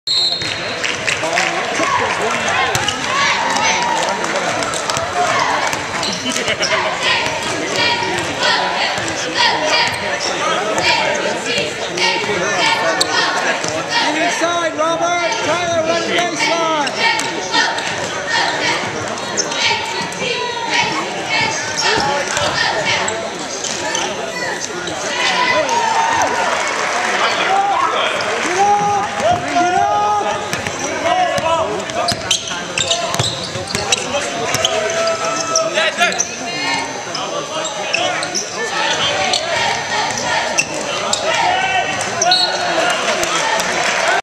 MacombWestern.MP4
Opening tip at Macomb-Western Holiday Tournament between Macomb and Barry Western.  Host Bombers open up with a three-pointer on their first posession.